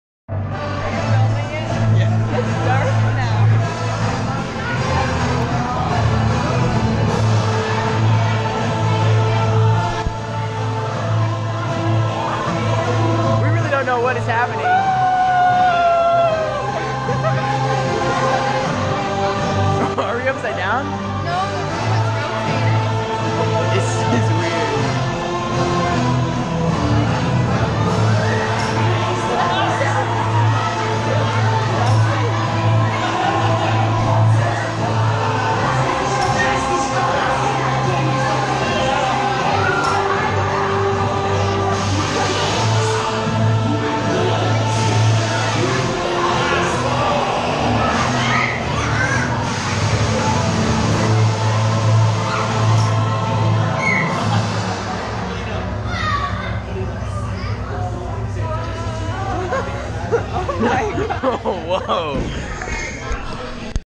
Strange Ride in Europa Park
We spend the day at Europa Park, Germany. In the Greece section, our exchange students lead us into a building without telling us anything. We go in an sit down in benches not knowing what will happen next. This is what starts happening.